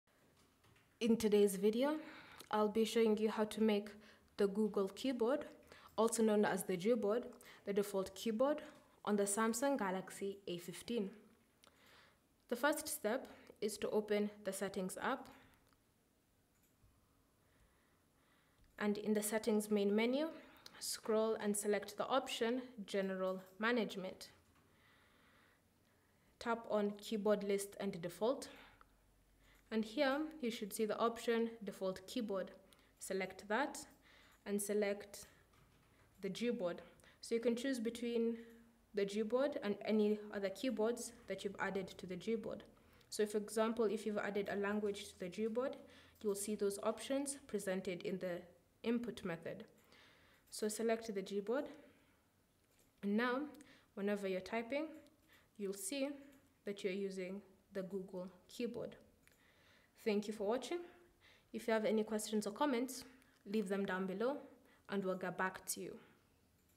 How To Make Gboard Default Keyboard On Samsung Galaxy A15
Learn How To Make Gboard Default Keyboard On Samsung Galaxy A15.

Are you tired of the default keyboard on your Samsung Galaxy A15 and want to switch to Gboard for a better typing experience? Look no further! In this tutorial, we'll walk you through the simple steps to make Gboard your default keyboard on your Samsung Galaxy A15.

Gboard, developed by Google, offers a wide range of features including predictive text, gesture typing, voice typing, emojis, GIFs, and more, making it a popular choice among Android users.

1. Open your phone's settings. Scroll down and select "General management."

2. Tap on "Keyboard List & Default." Then select Default Keyboard.

3. Choose the Gboard. Once selected the changes will be applied automatically.

Cell Phone Tripod Adapter